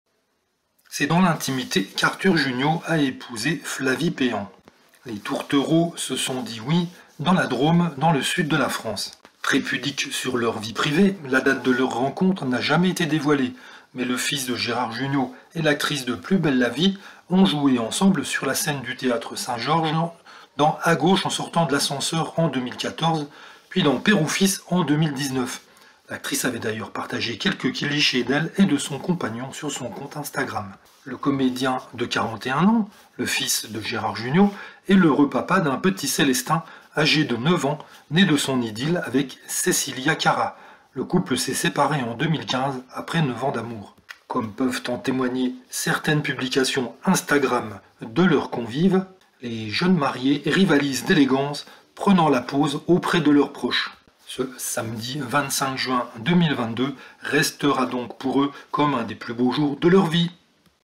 Gérard Jugnot au mariage de son fils Arthur Jugnot et Flavie Péan
L'acctrice de plus belle la vie s'est mariée dans la Drôme le samedi 25 juin avec le papa du petit Célestin